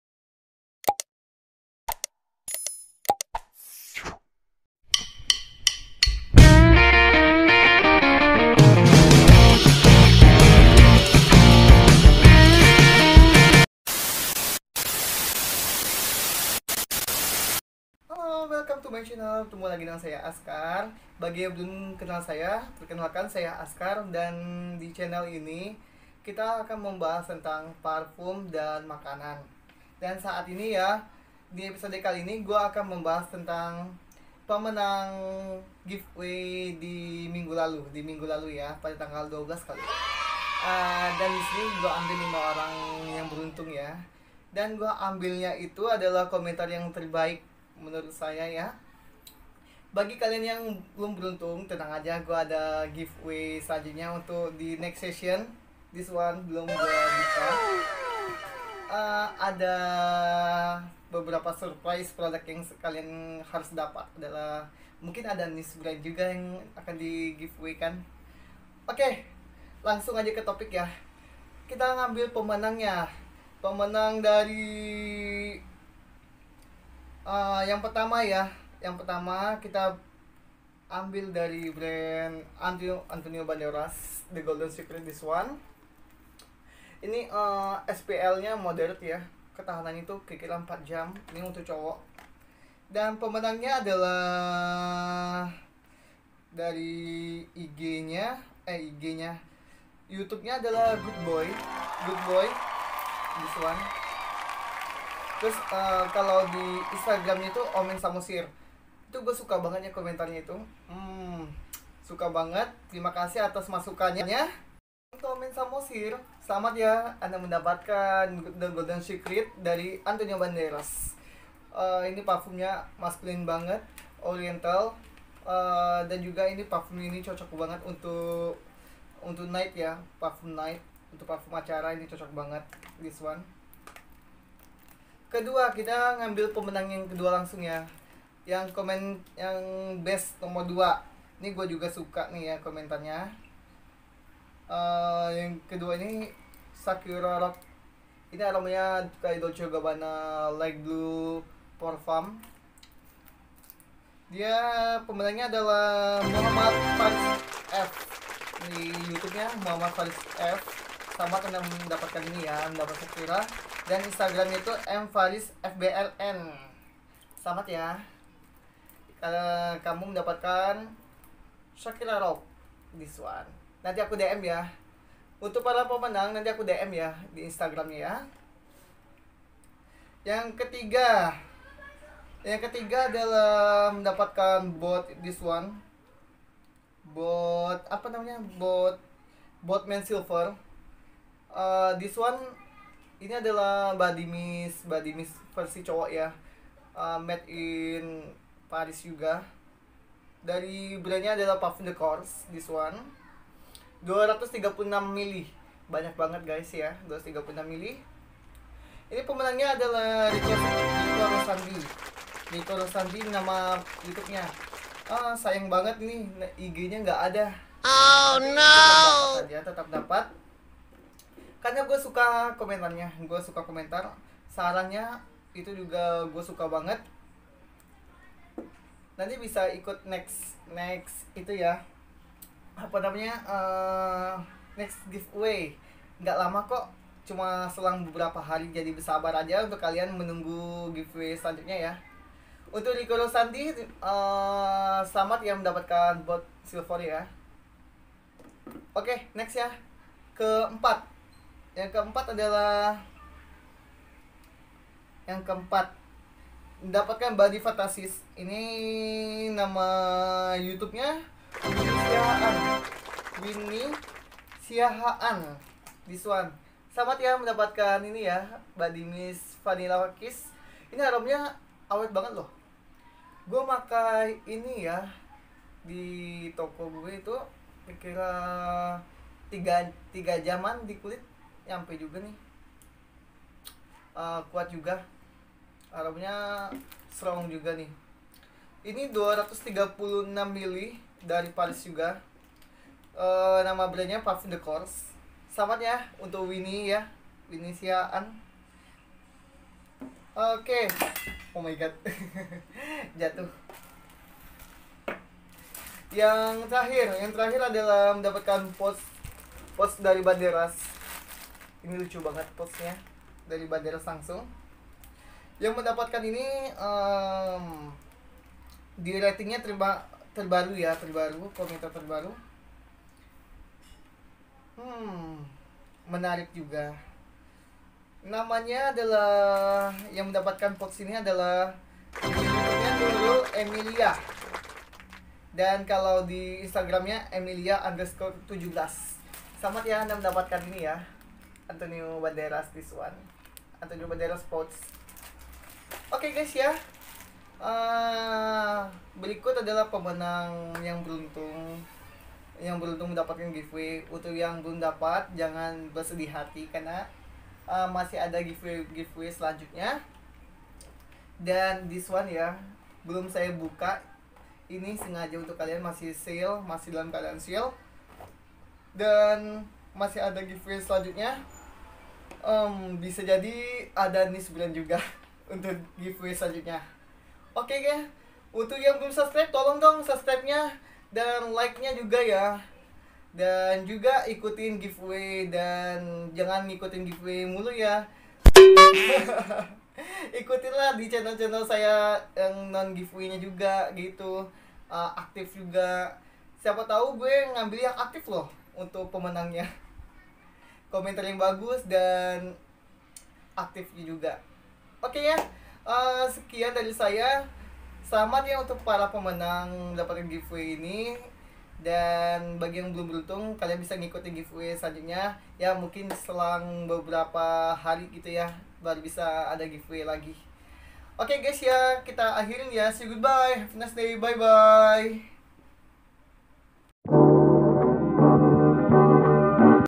Siapa kah pemenang Giveaway ??? Tonton sampai abis yaa
Haloo guys..
Nah hari ini adalah hari yg kalian tunggu ,
Diepisode kali ini akan dinobatkan siapa yg beruntung mendapatkan beberapa giveaway 👏👏..
Untuk yg belum beruntung bisa ikutan lagi di giveaway akan mendatang kisaran beberapa hari lagi hehe , bagi yg udah pernah menang bisa ikutan lagi yaa..
Dan juga kepoin juga yaa Beauty & Scent disana kalian menemukan parfum dan cosmetic yg kalian cari😍
Saya harap kalian ikutin channel saya terus yaa.
Untuk pemenang hari ini akan saya DM malam ini😍😍, ditunggu yaaa..